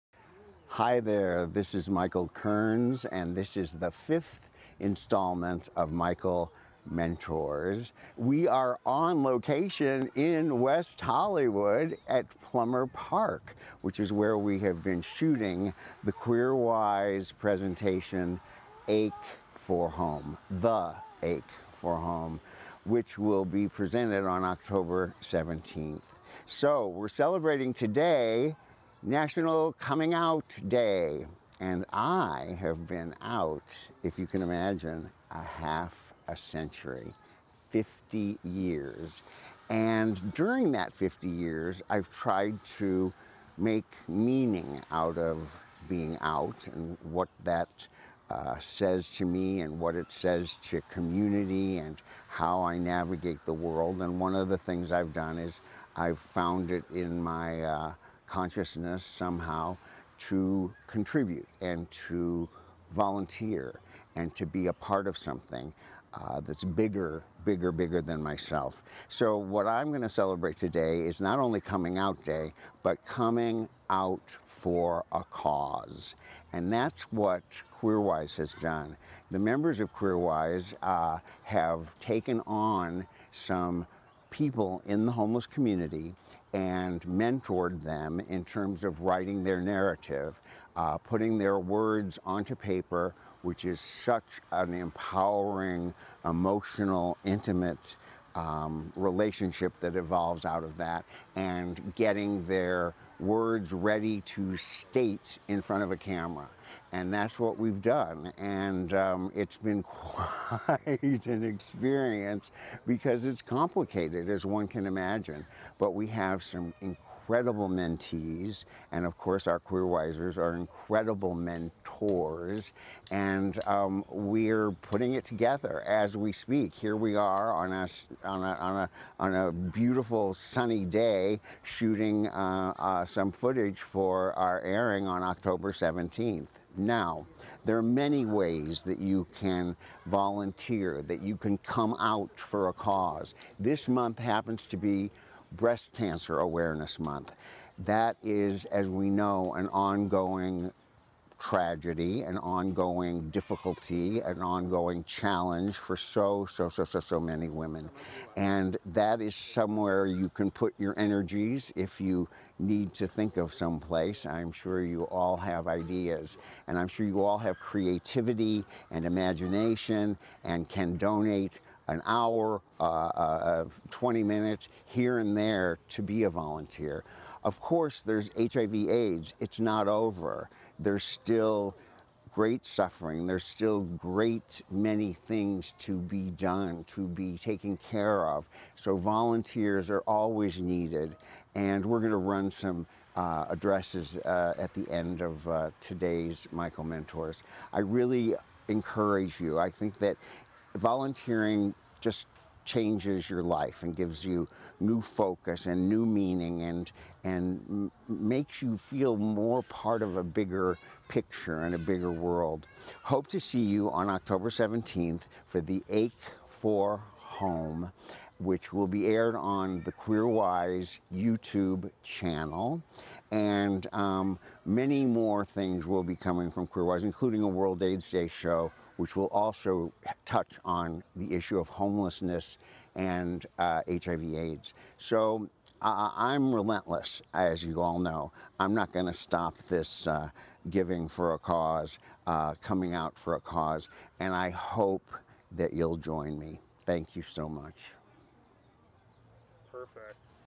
Michael Mentors EP 5: Come out today with a cause
Michael Kearns, Hollywood's first openly gay actor, celebrates National Coming Out Day and his 50th anniversary of being out, with a conversation about coming out with a cause.